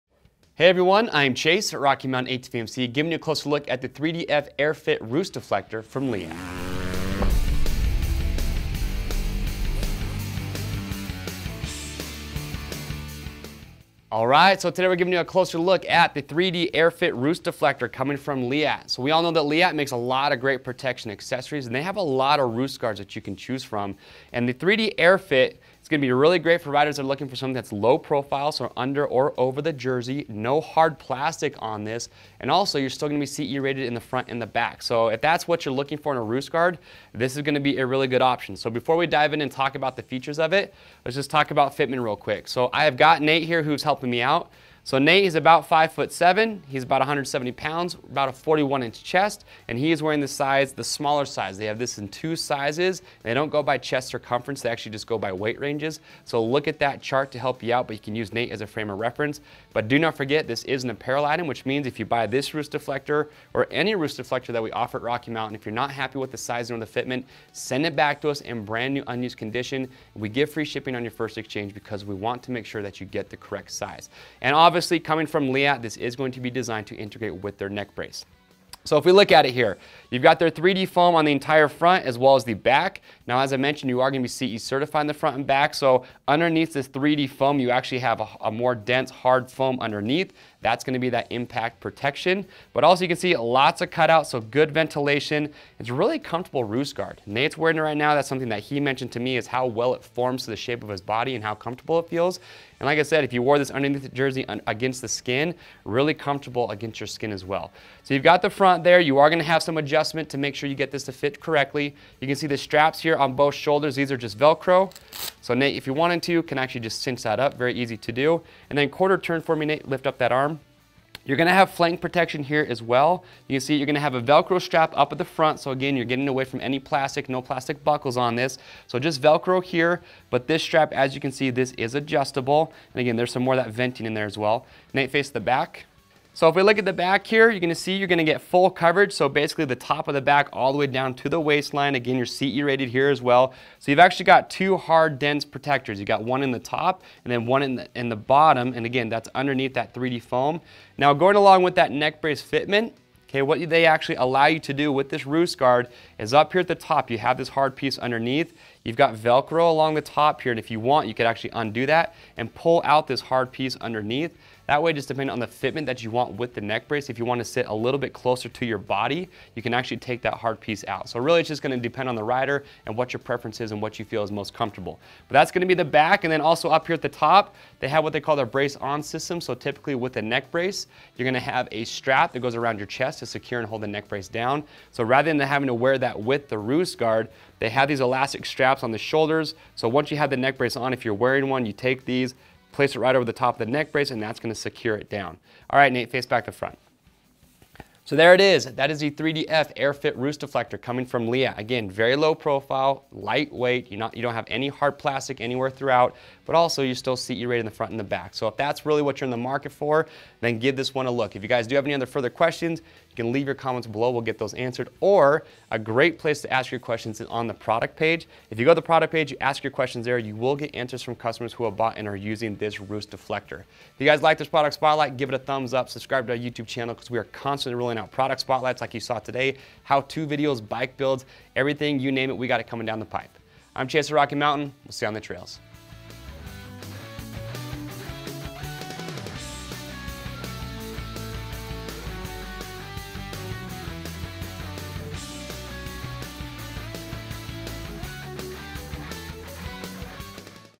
Leatt 3DF AirFit Motocross Roost Deflector
The Leatt 3DF AirFit Roost Deflector offers ventilated soft impact foam.Multi-layer multi-plate articulating design. 3D design ensures a great fit that is very comfortable. Ventilation slots for maximum airflow.  Removable front & rear plates allow over / under the shirt Leatt neck brace fit.  Adult size fits riders from approx. 150 to 198 lbs.  Adult XXL size fits riders approx. 198 to 286 lbs.